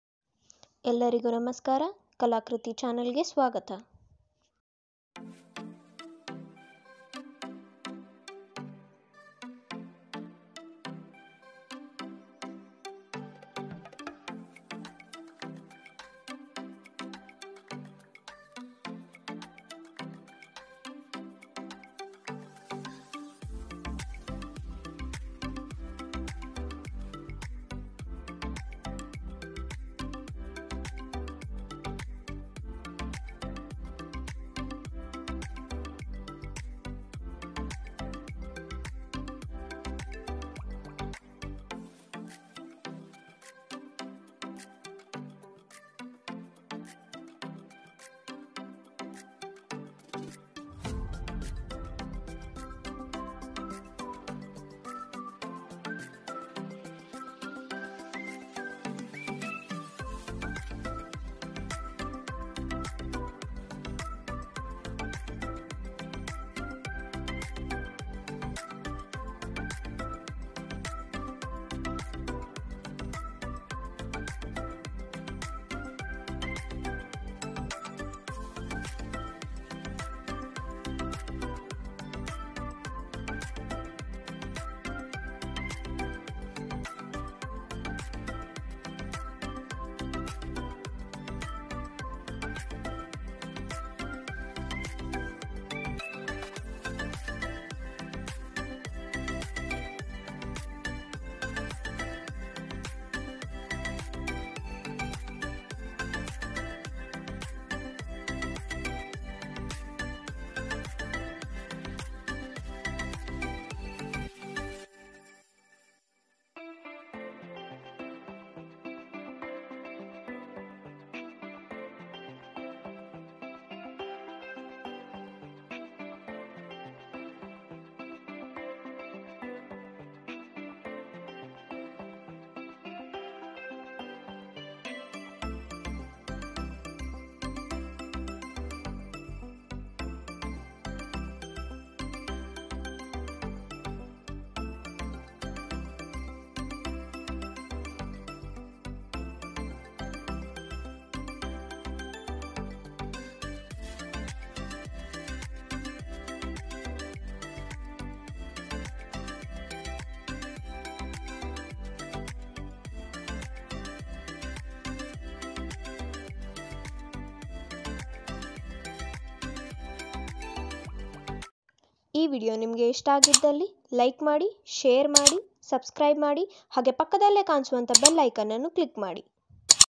DIY|how to make paper rose|5 minutes easy craft
DIY|how to make paper rose|5minutes easy craft|easy DIY paper craft

materials required
-colored paper
-scissors
-glue